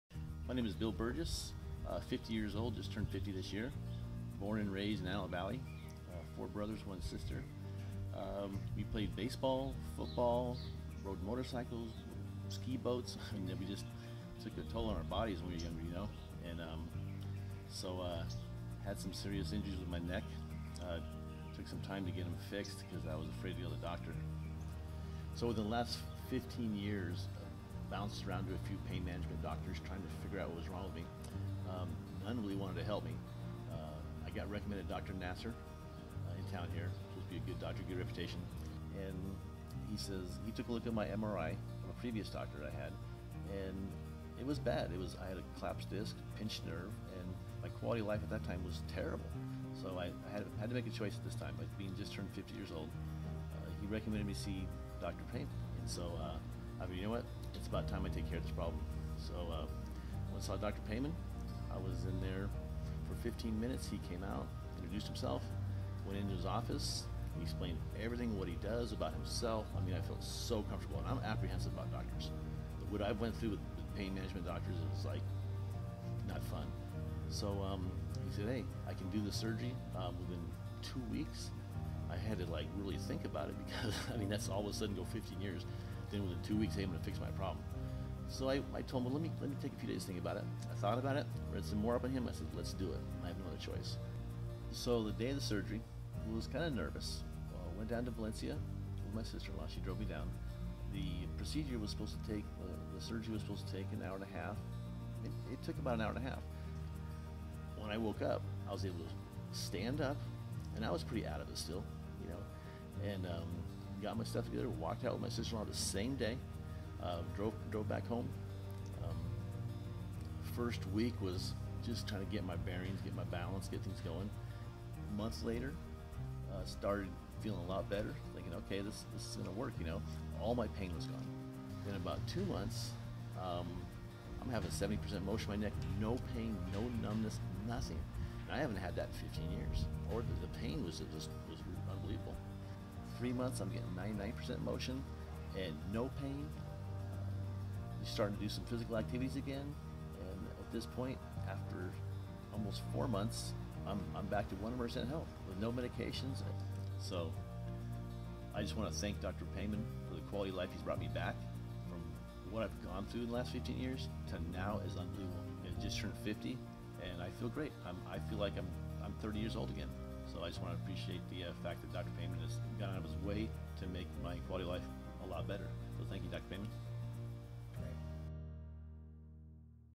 Bill
Bill, will tell you about his experience with Dr. Payman